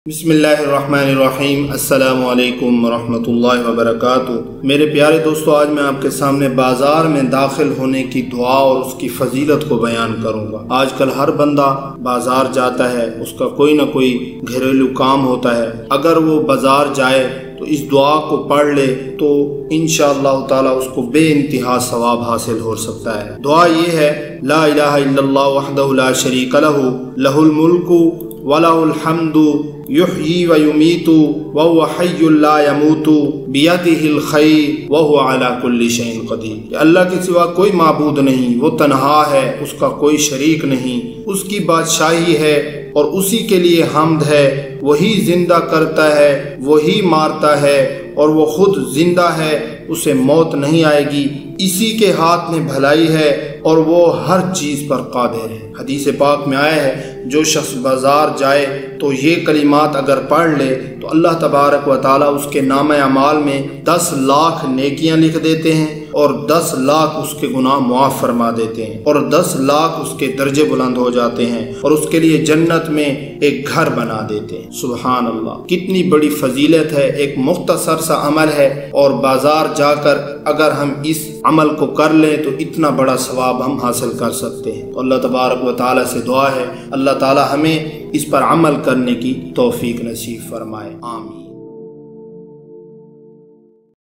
Bazar Me Jane Ki Dua Aur Uski Fazilat | Bazar Mein Dakhil Hone Ki Dua
Aj Ki Video Main Ap Ke Samne Bazar Me Jane Ki Dua Aur Uski Fazilat Dua When Going To Market Bazar Mein Dakhil Hone Ki Dua Ko Bayan Karin Ge To Is Ke Liye Aj Ki Is Video Ko Akhir Tak Lazmi Dekhain. Thanks For Watching...